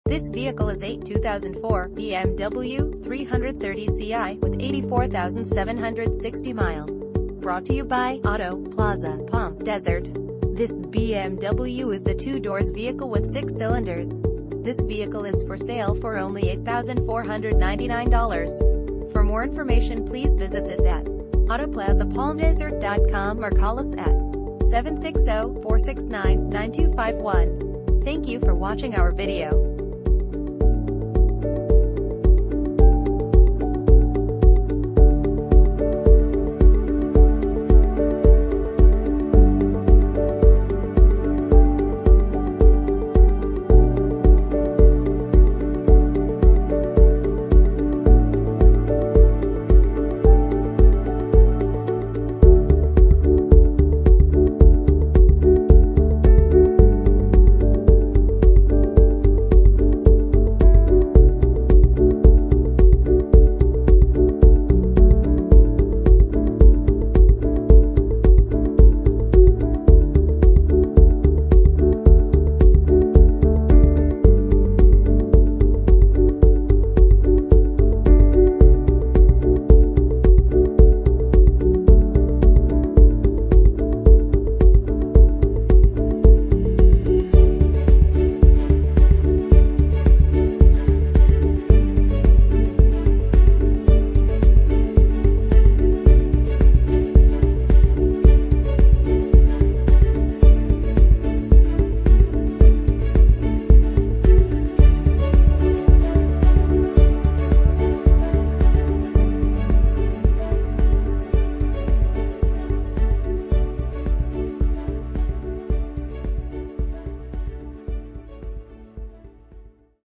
2004 BMW 330Ci - Auto Plaza Palm Desert
Auto Plaza Palm Desert
41700 Corporate Way Suite A

Clean title, 84,760 miles, Automatic, Alpine White exterior, Sand interior, Convertible, RWD: Rear Wheel Drive, 2 doors, 6 cylinders, Gasoline, MPG City 19 and 28 in Hwy.